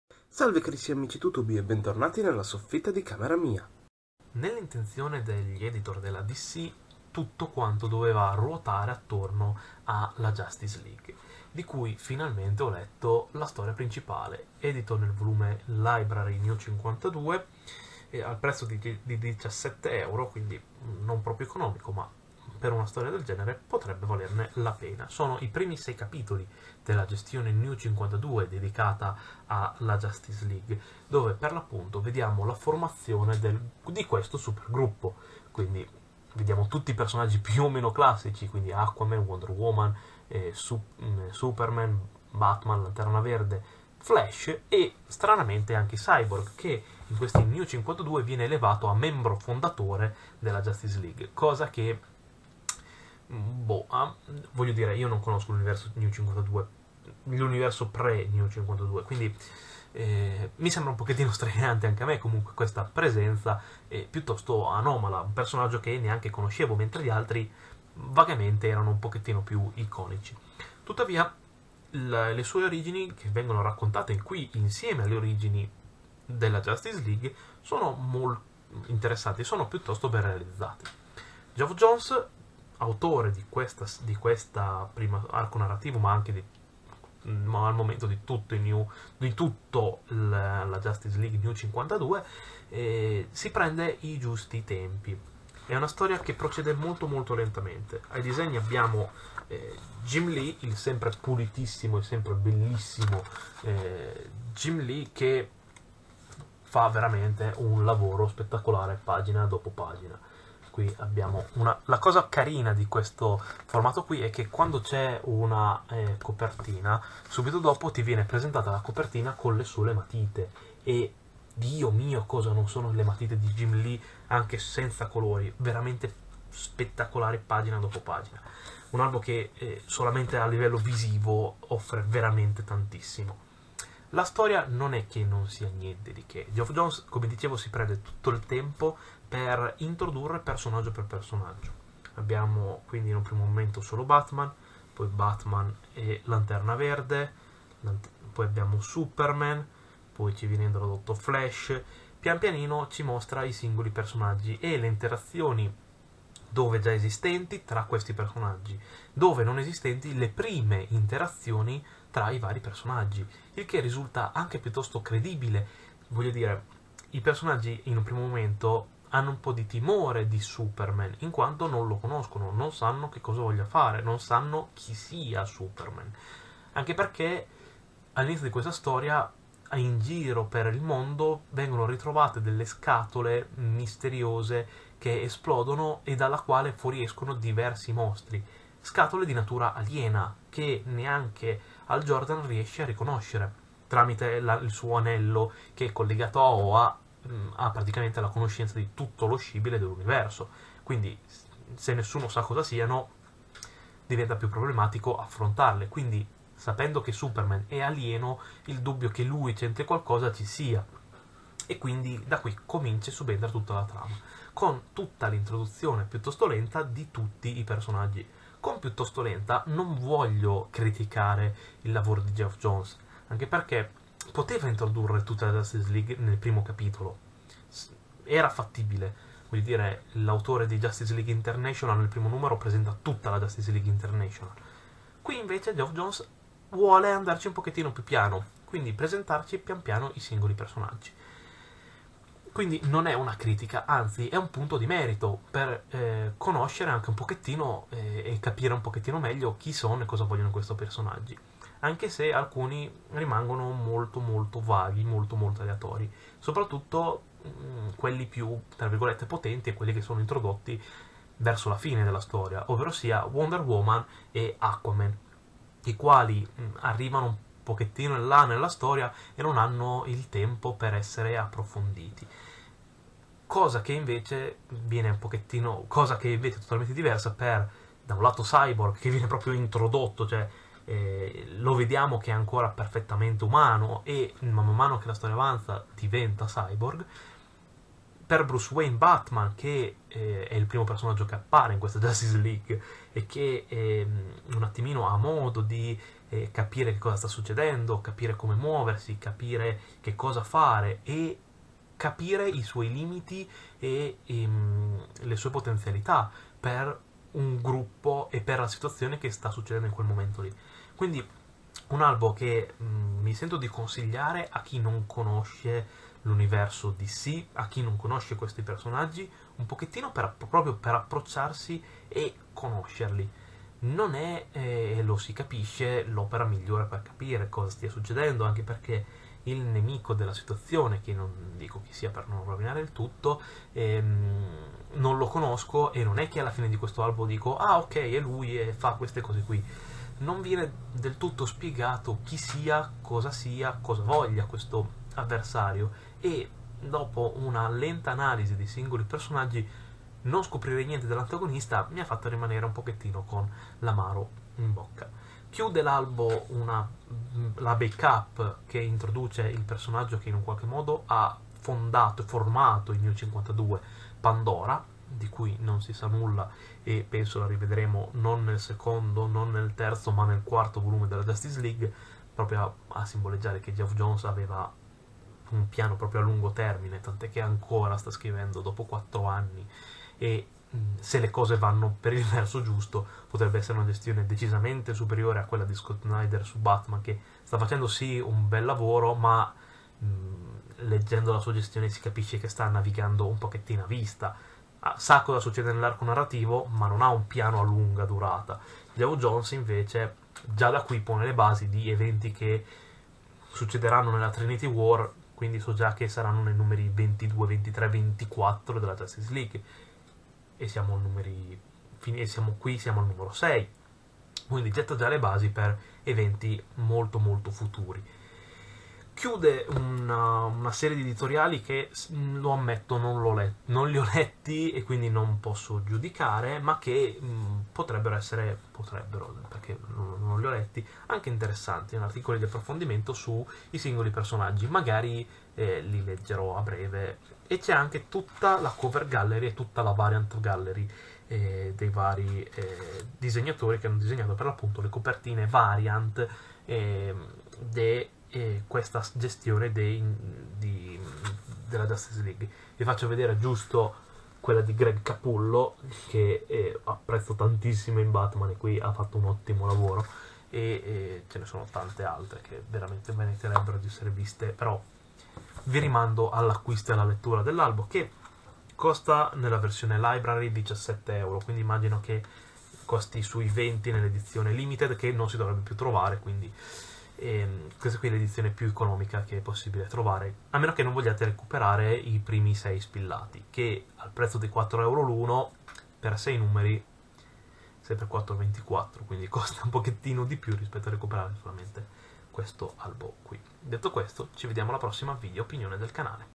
[Nuovi 52] Justice League Origini
Geoff Johns e Jim Lee ci introducono la nuovissima Justice League all'interno dell'iniziativa editoriale New 52.
Cosa ne penso di questa prima storia? Scopriamolo insieme in questa nuova video opinione del canale.